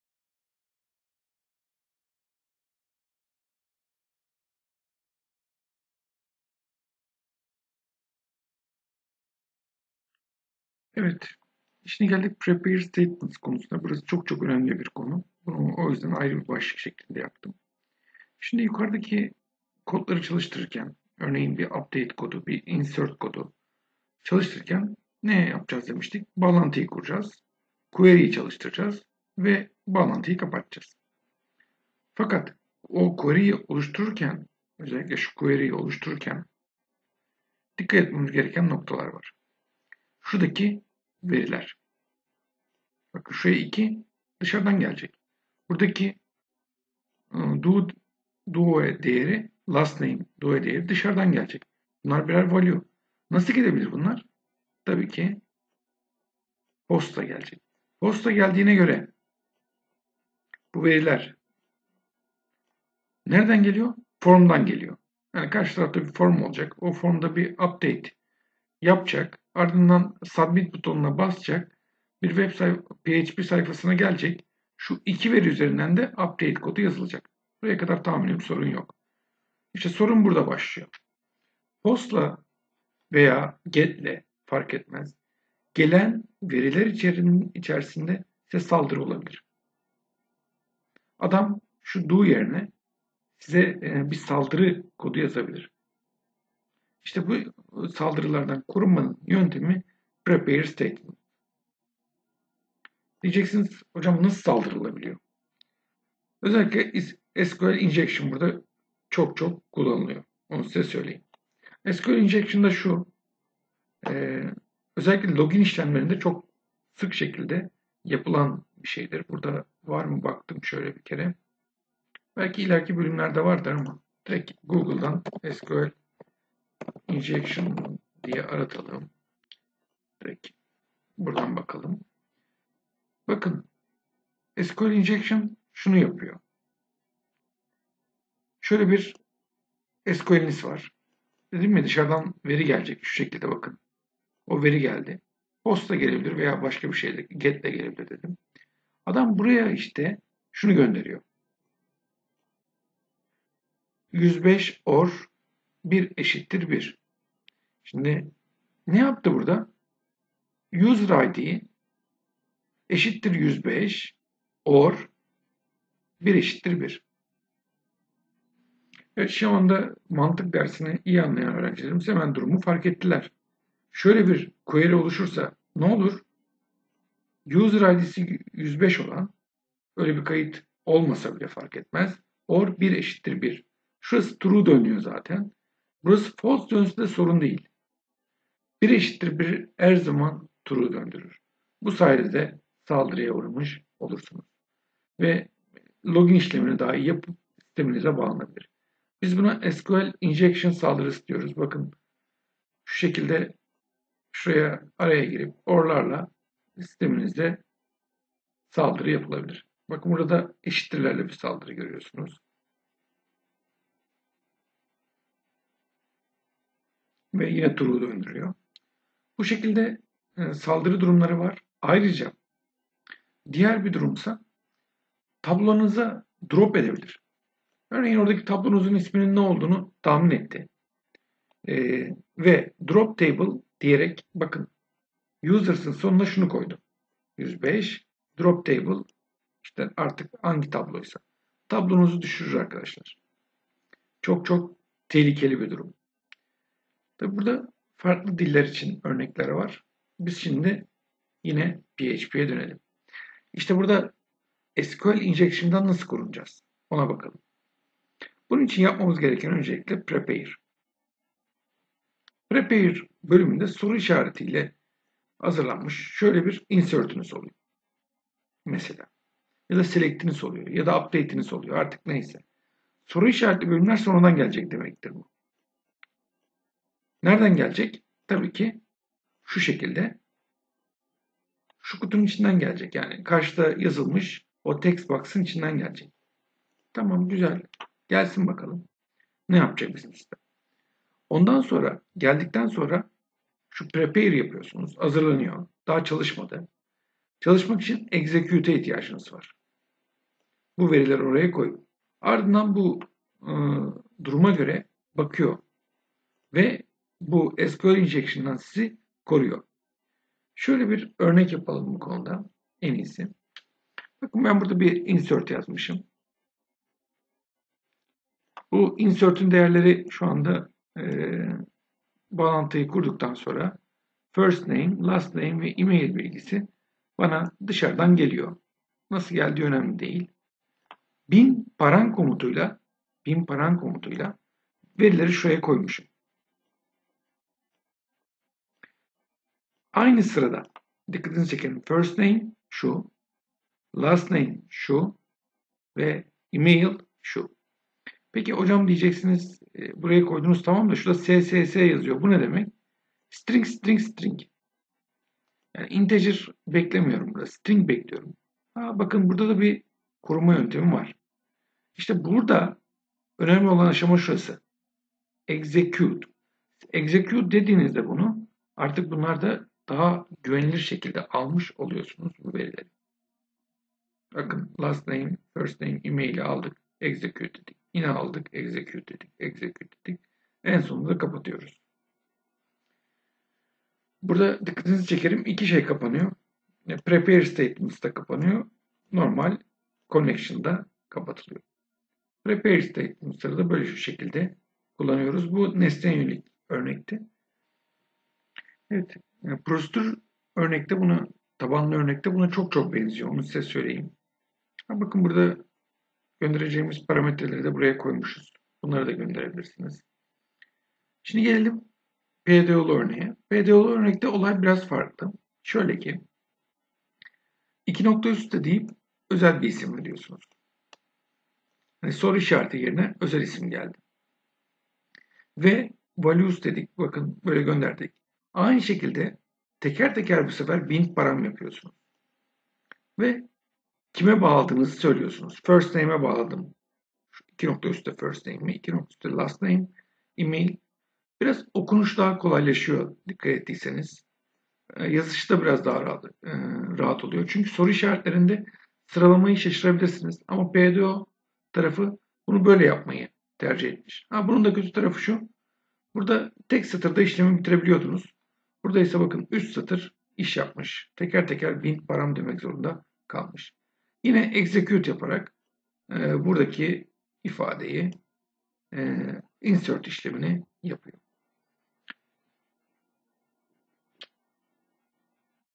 WP 08.01.03 PHP - MySQL: Prepared statements
PHP'de güvenlik açısından önemli Prepared statements konusu anlatılmıştır.